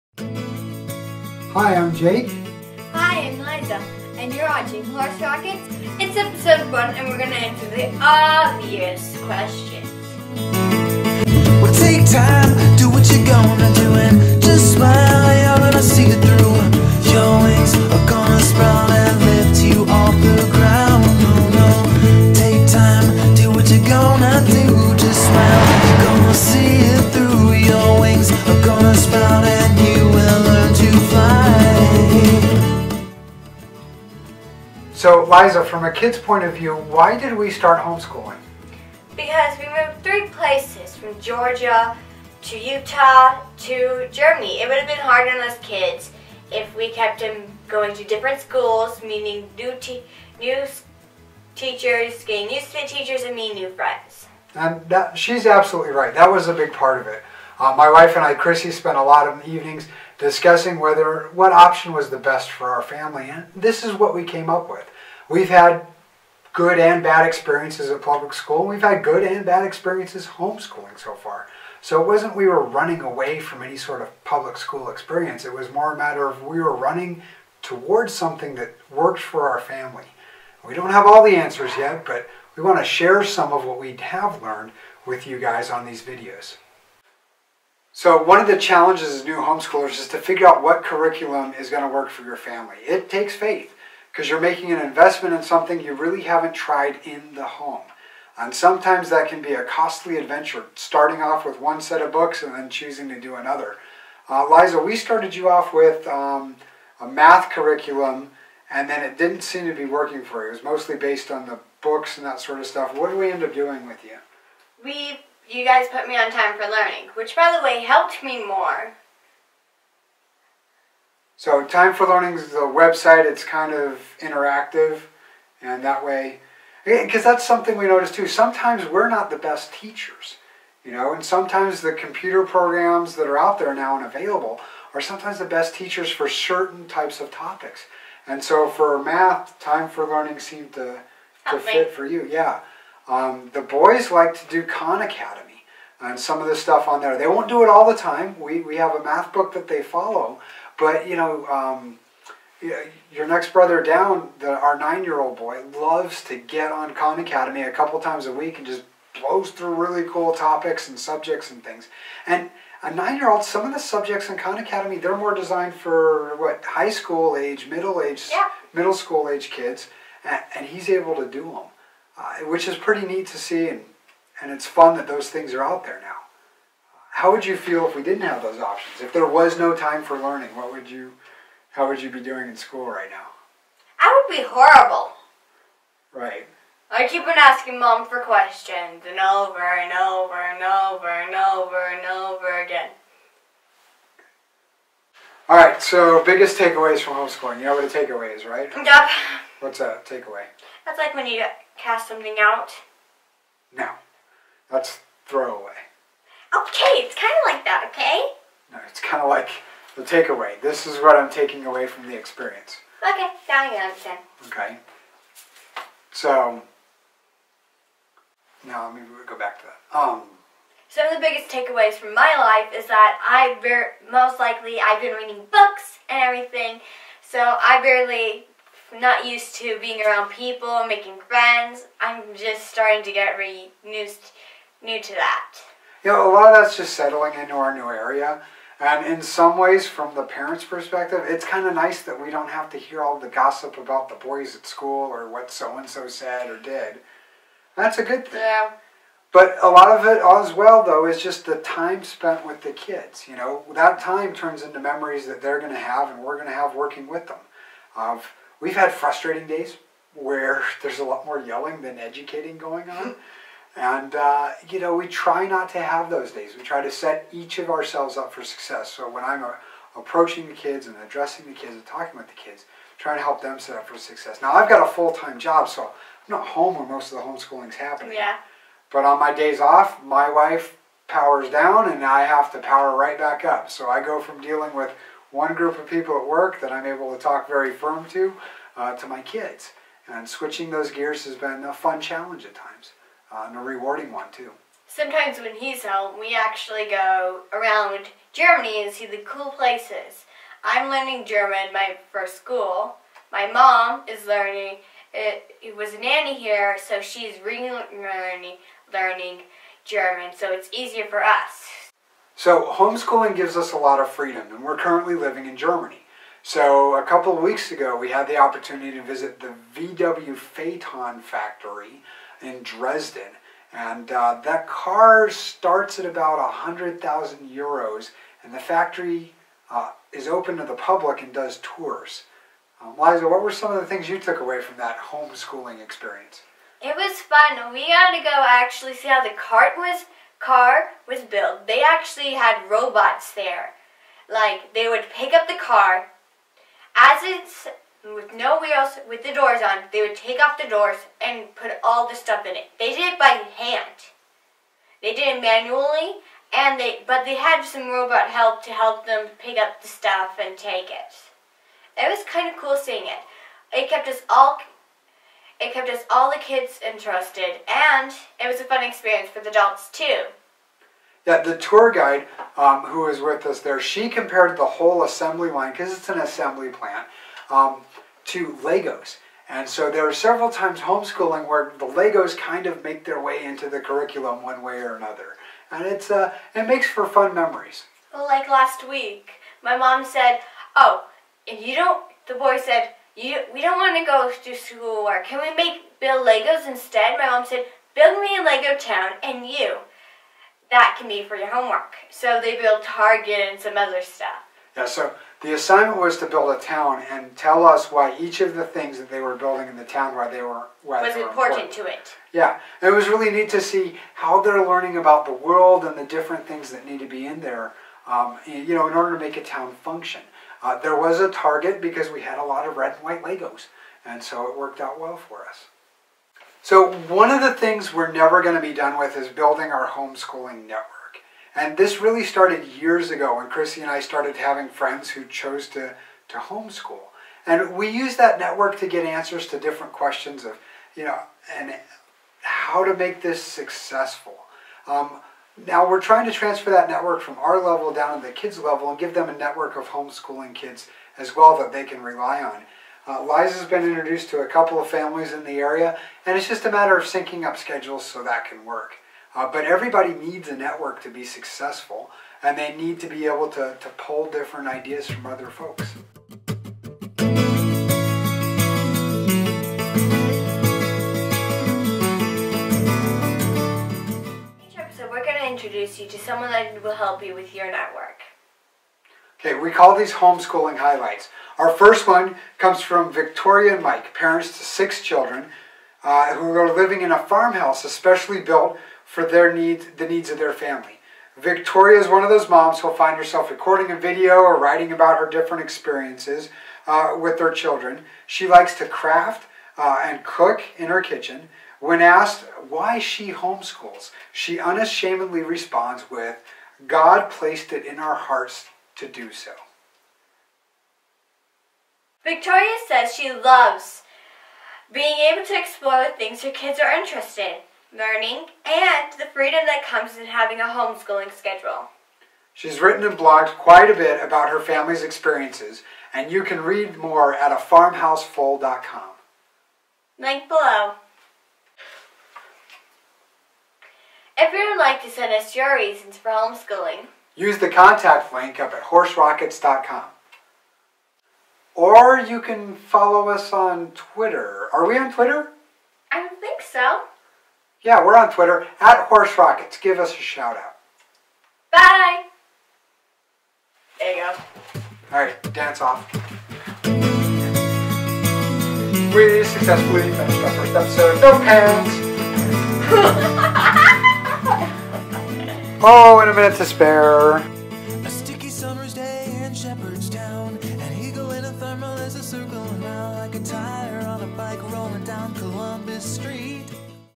Horse Rockets 1 The Obvious Questions
How did we start homeschooling?
How did we choose the curriculum?
What are the biggest take aways so far from your Homeschooling experience?
How did we build our network?